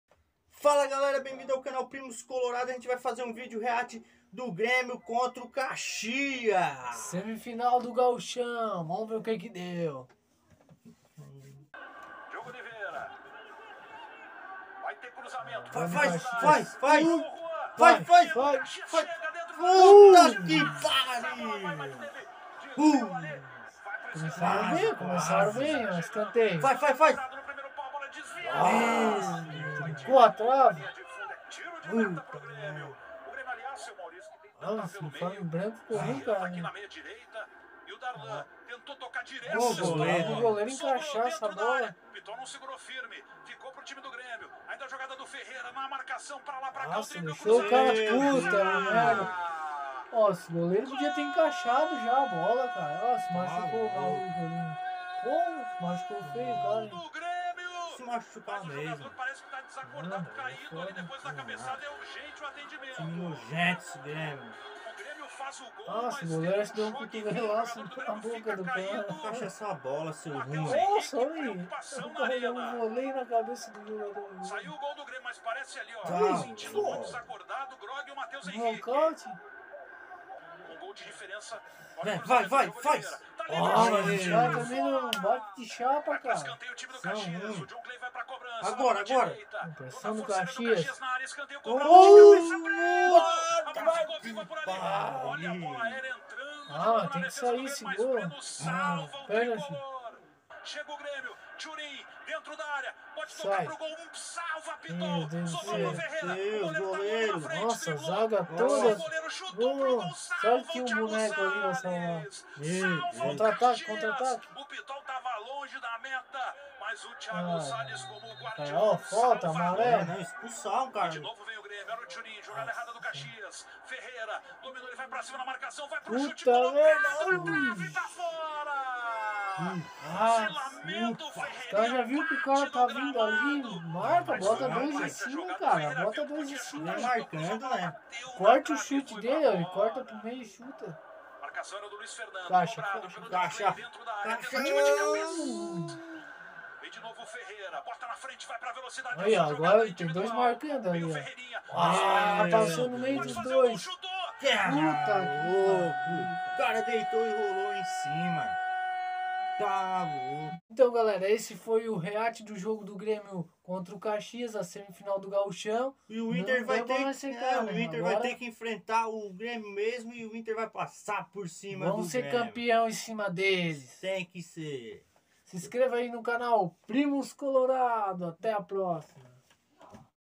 😡REACT:SECANDO O GRÊMIO NA SEMI FINAL CONTRA O CAXIAS NO CAMPEONATO GAÚCHO 2021!
COLORADOS SECANDO O GRÊMIO NA SEMI FINAL DO GAUCHÃO
GRÊMIO É FINALISTA E VAI ENFRENTAR O INTER NA FINAL!